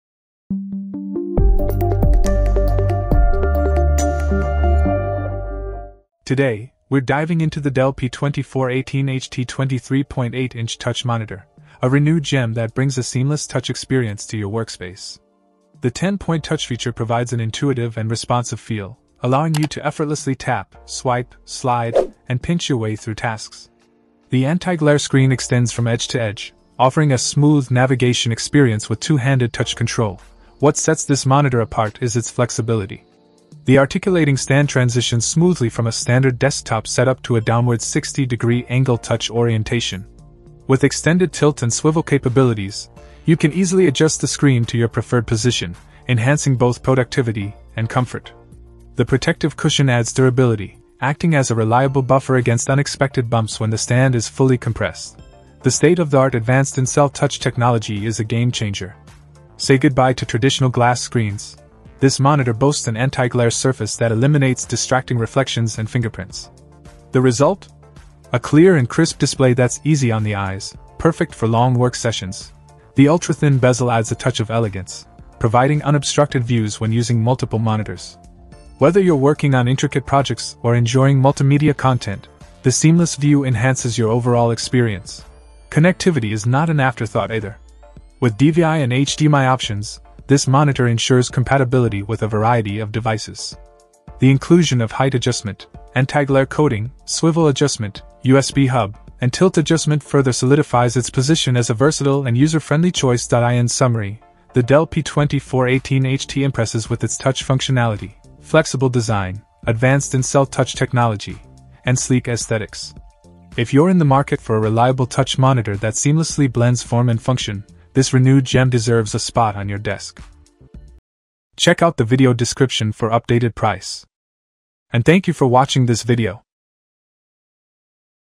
Dell P2418HT 23.8in Touch Monitor - In-Depth Review
0:00 Intro
0:06 Review

Things that we mentioned in this video:
Dell P2418HT 23.8in Touch : B07P7LWFJT